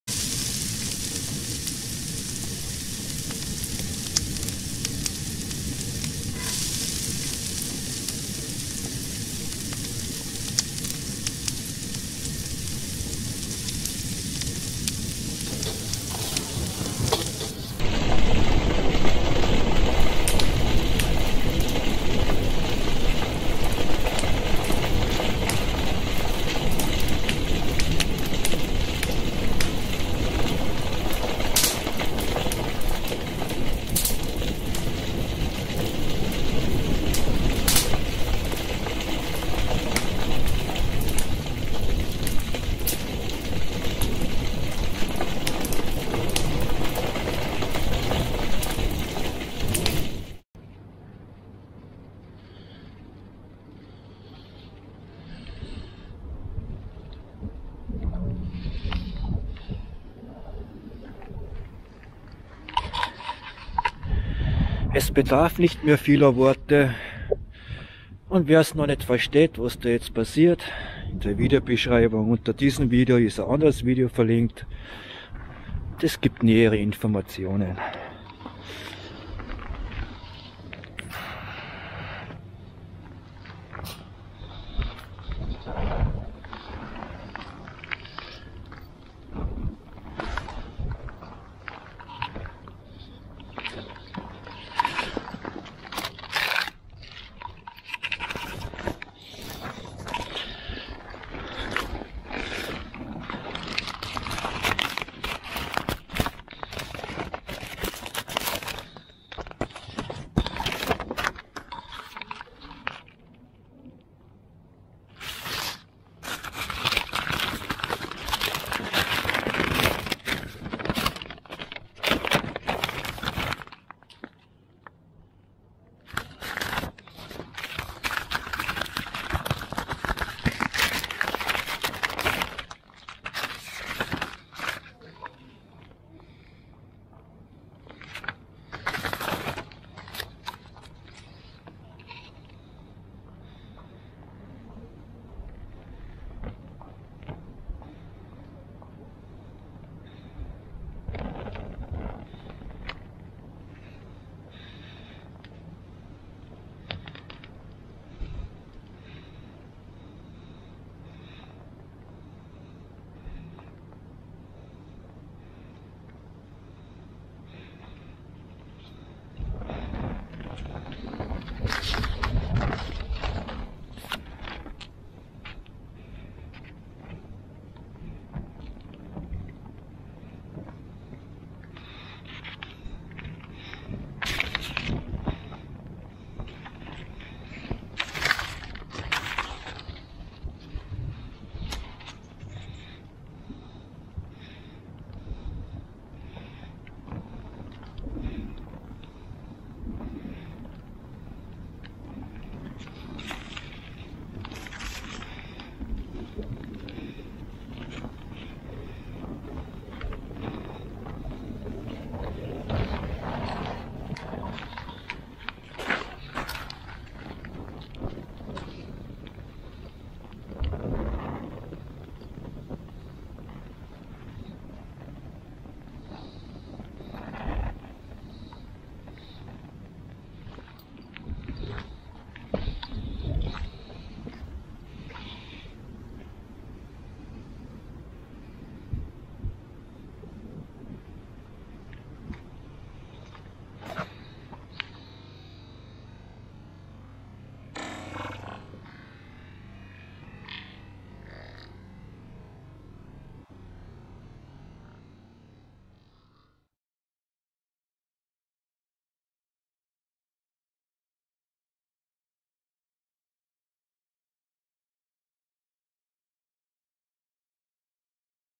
Bücherverbrennung am 30 April 1938 in Salzburg im vergleich zum Heutigen Zeitgeist.
Video wo ich erkläre warum ich meine Anzeigen & Strafverfügungen Verbrenne:
Am besten "BRENNEN" die Mahnbriefe meiner Strafanzeigen - TOTALER WIDERSTAND!